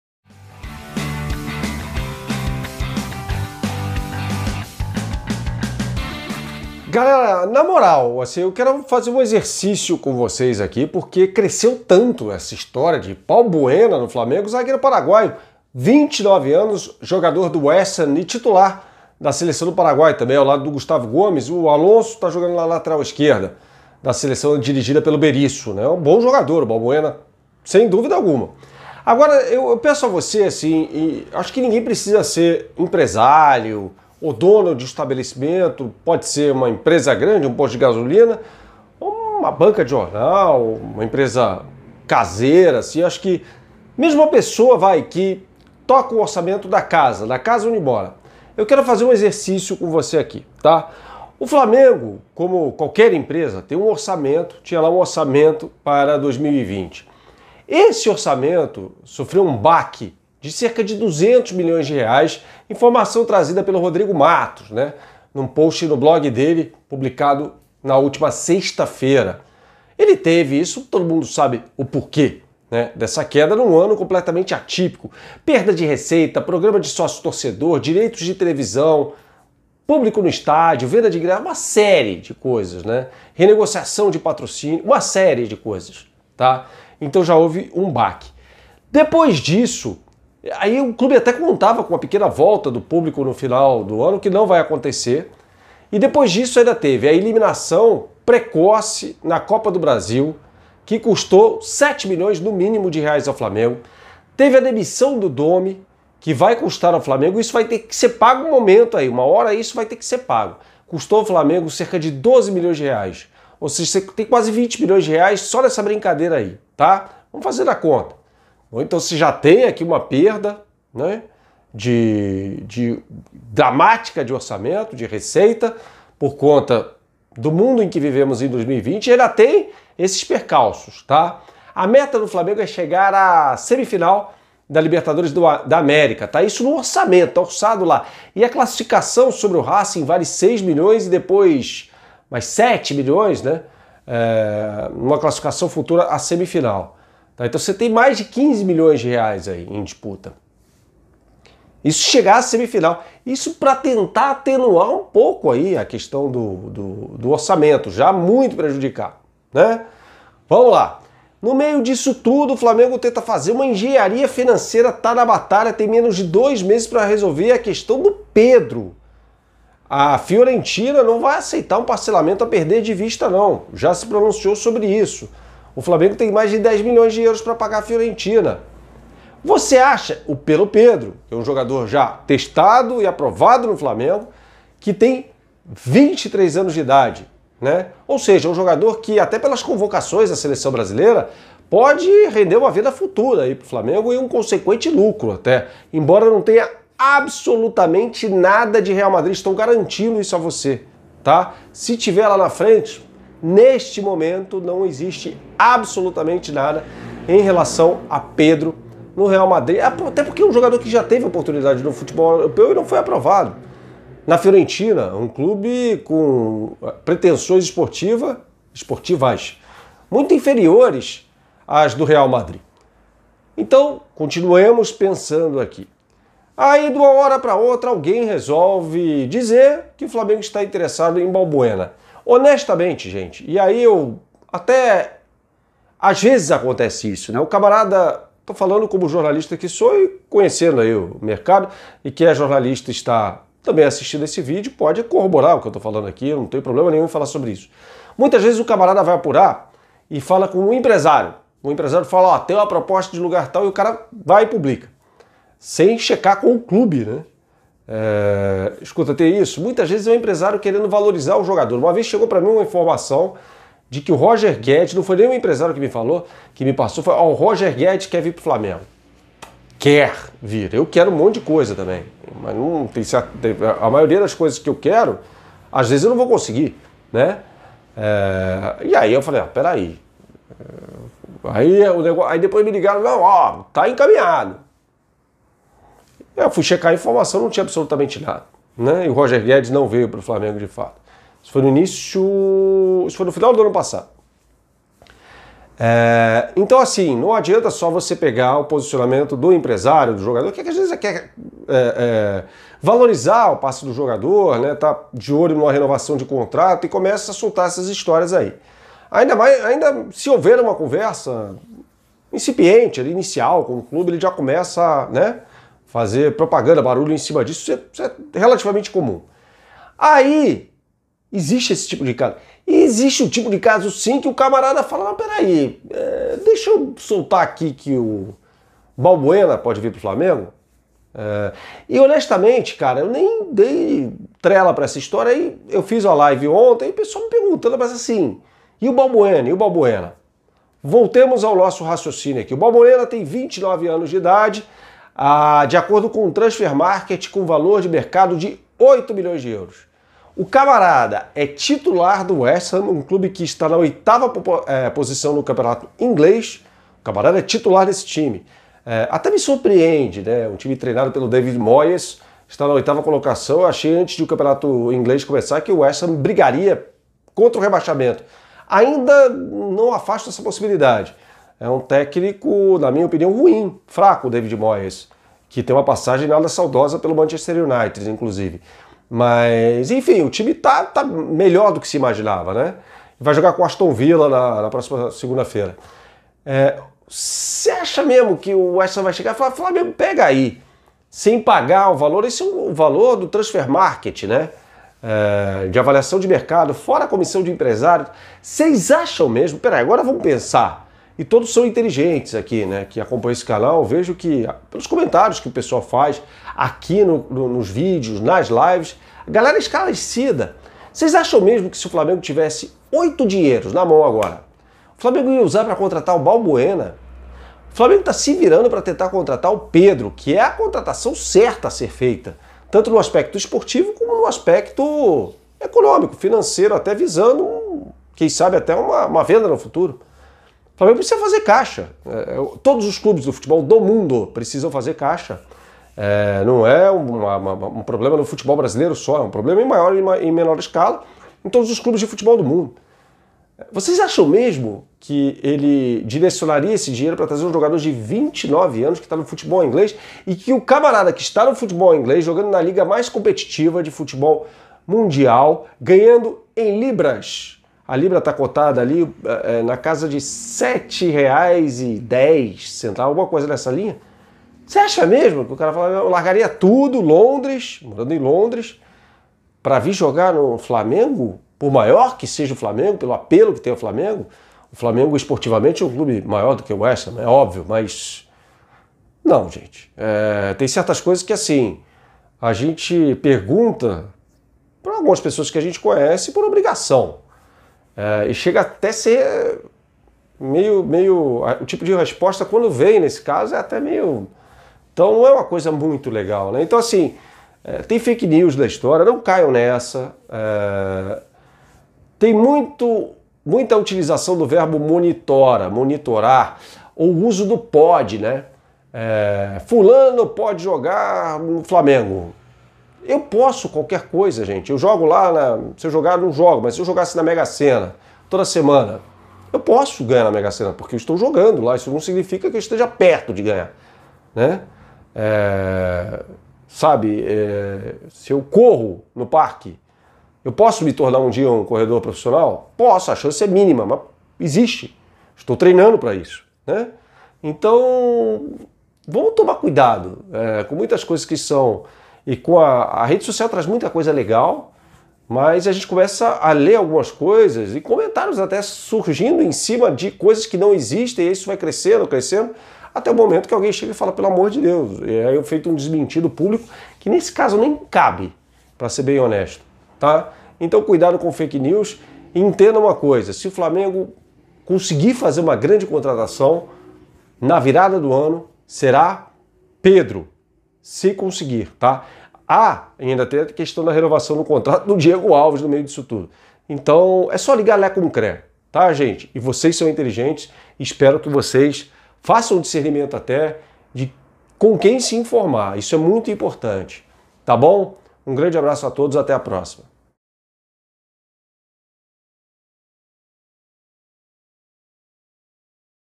Com oito milhões de euros, Flamengo compraria Pedro ou iria atrás de Balbuena? Não caiam nessa
O Flamengo, como qualquer outro clube do mundo, vive um período de contenção de gastos, por motivos óbvios.

A direção rebola para juntar dinheiro para comprar Pedro. Por que faria uma aventura para contratar Balbuena, titular do West Ham, oitavo colocado Campeonato Inglês?